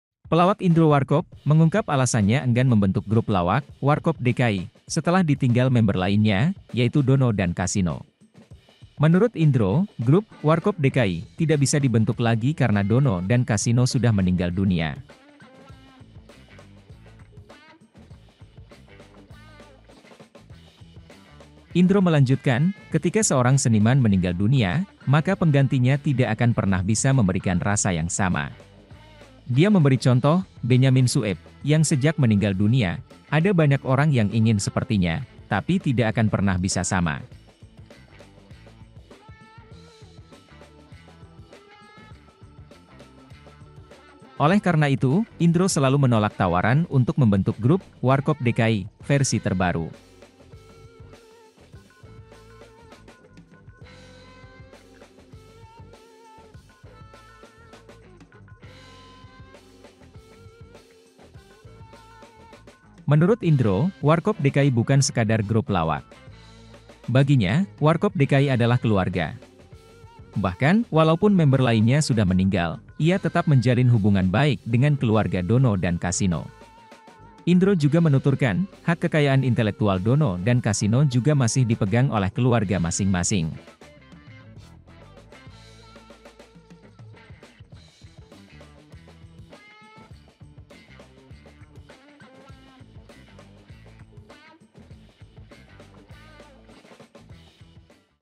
Enggan Bikin Grup Lawak Baru, Indro Warkop: Enggak Ada yang Bisa Gantiin
Pelawak Indro Warkop mengungkap alasannya enggan membentuk grup lawak Warkop DKI setelah ditinggal member lainnya, Dono dan Kasino. Menurut Indro, grup Warkop DKI tidak bisa dibentuk lagi karena Dono dan Kasino sudah meninggal dunia.